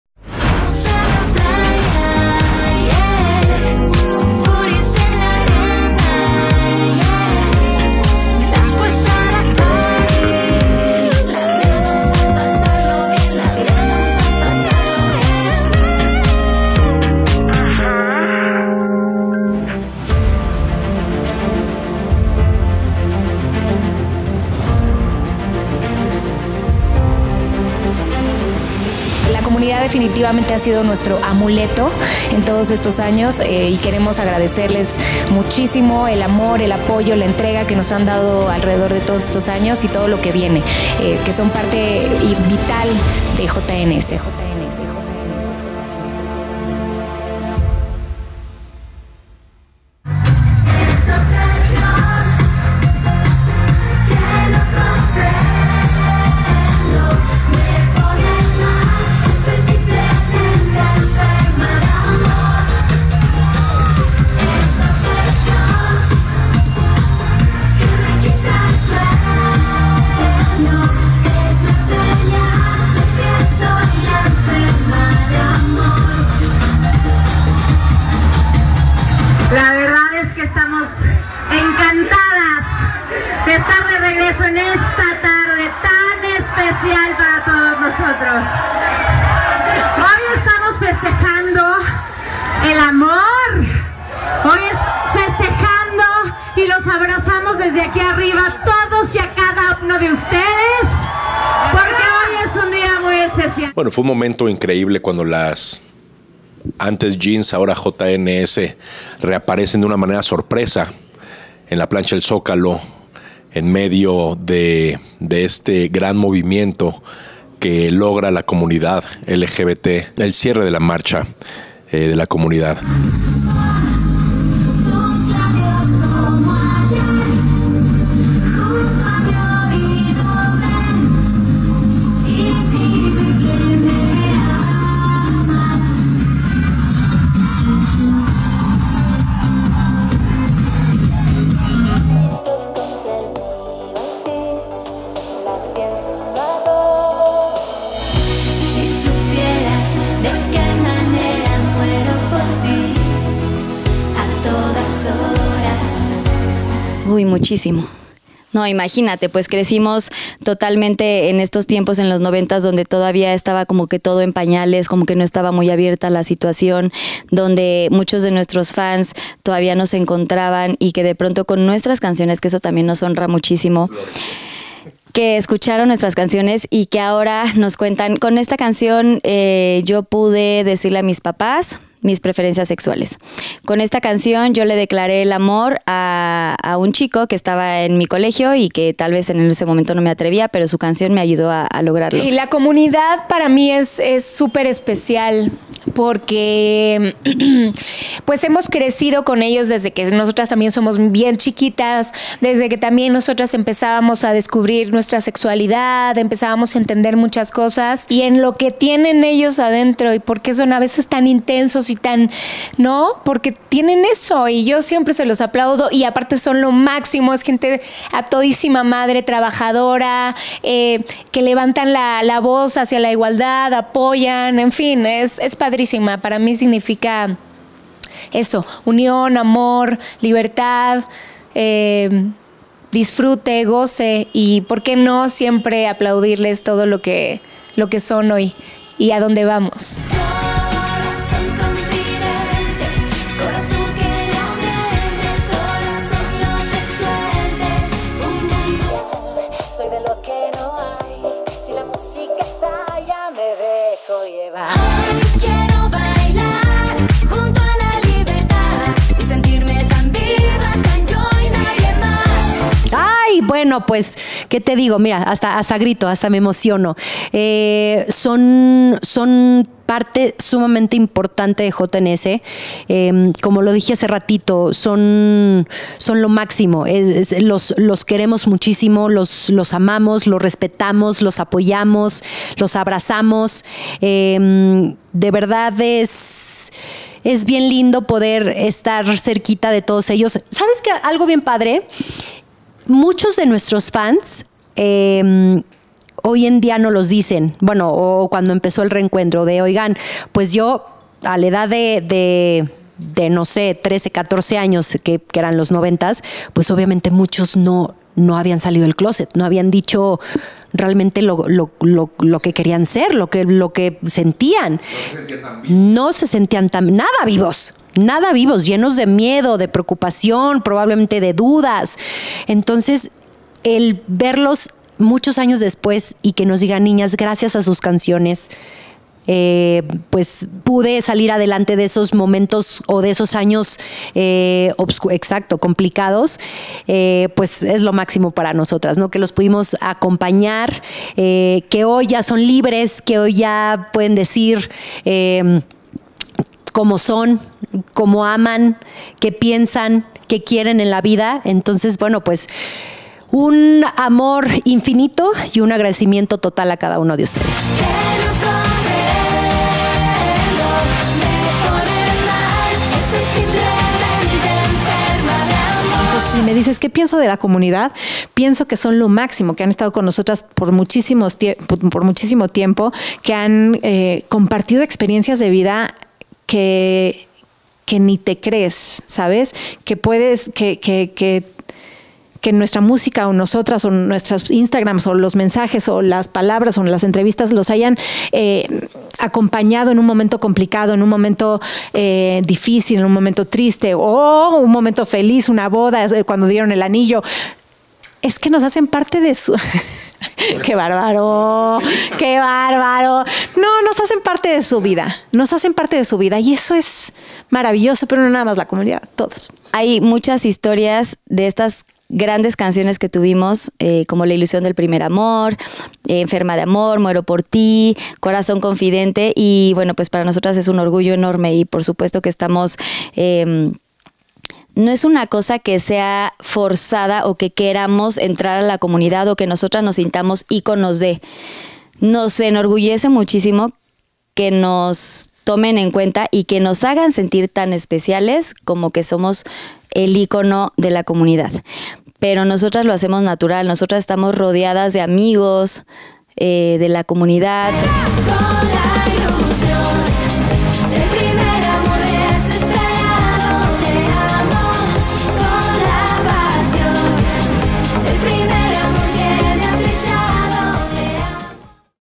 JNS, EL CONFESIONARIO CAP. 1 “AMOR ES AMOR” 🏳️‍🌈🌈
Angie, Melissa, Karla y Angie abren su corazón y hablan respecto al gran amor y las historias que les unen a la comunidad LGBT